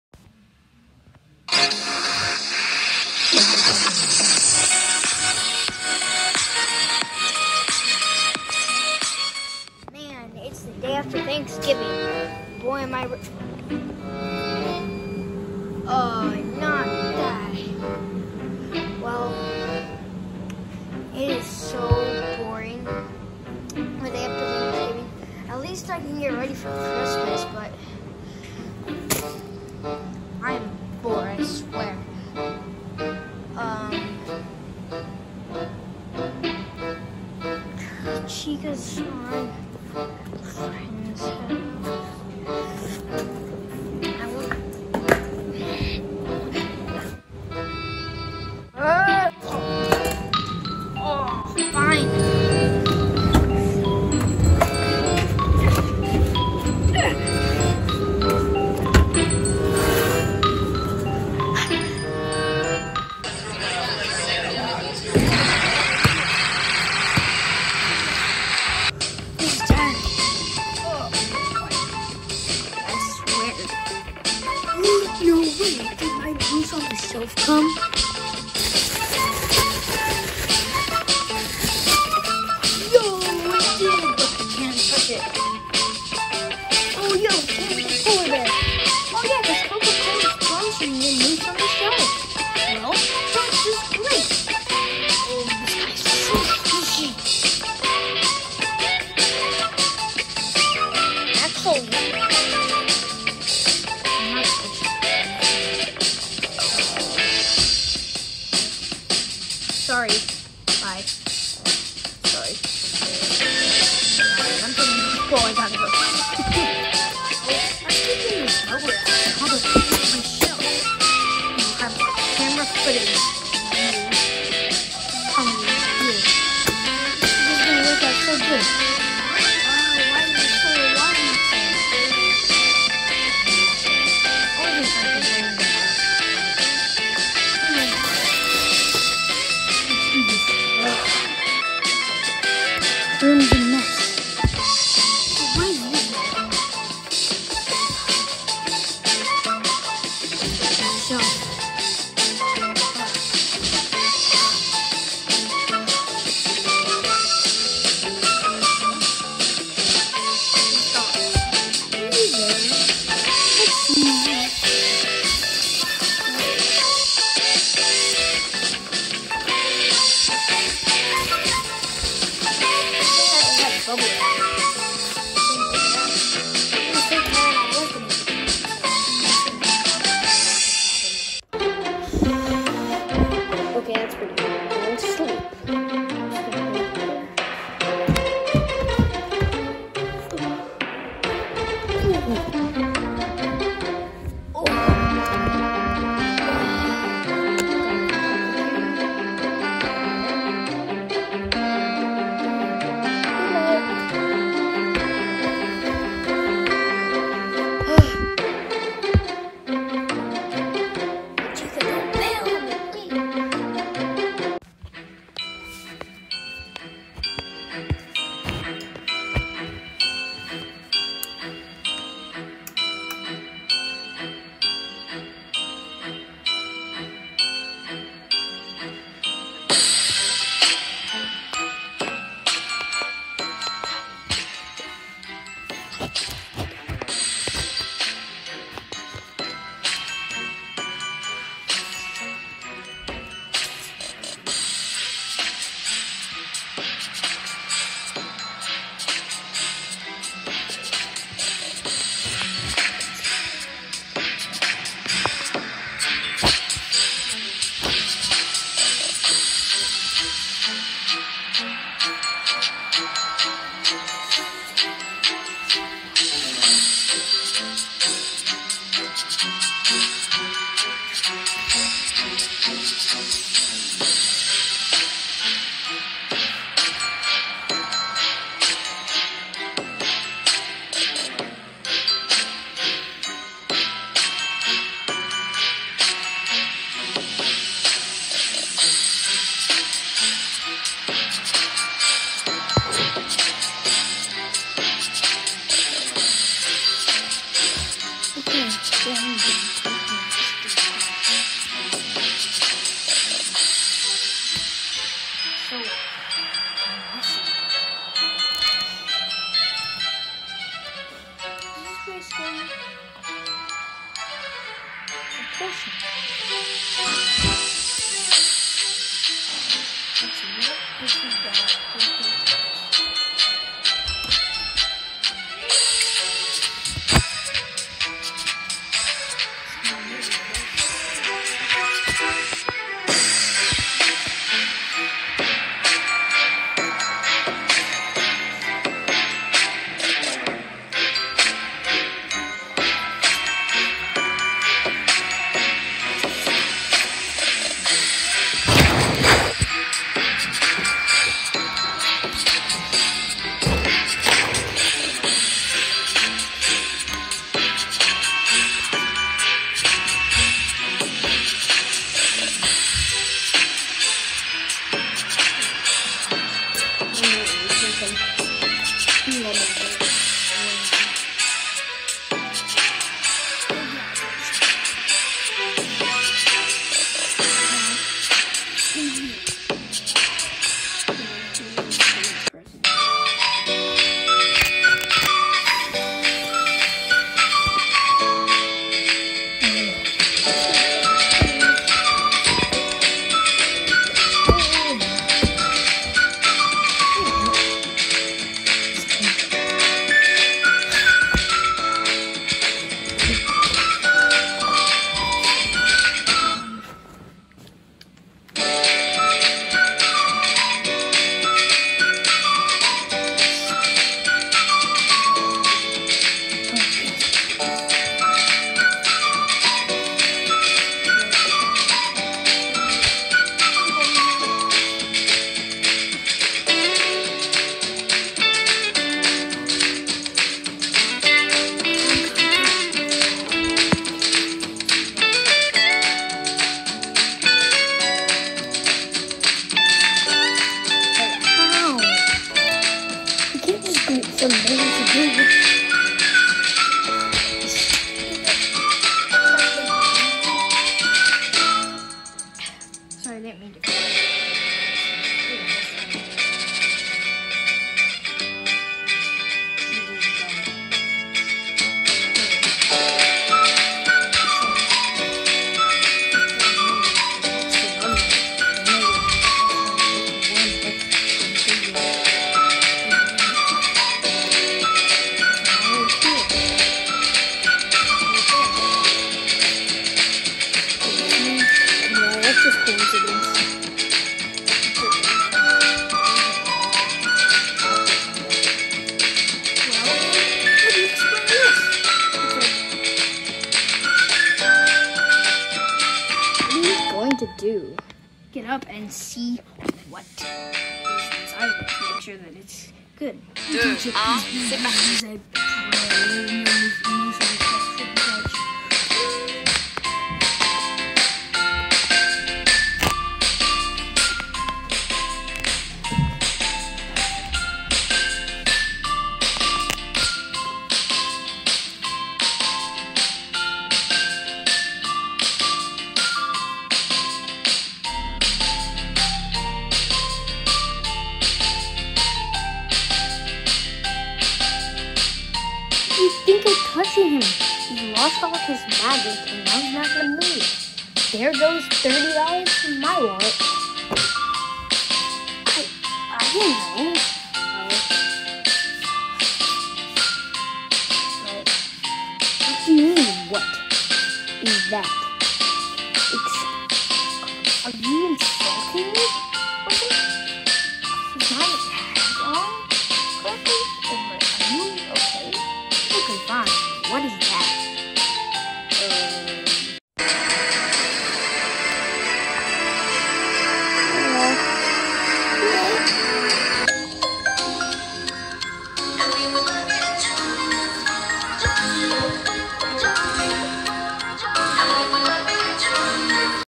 FNaF Plush: The Moose on the Shelf
Foxy gets a moose on the shelf and crazy things happen!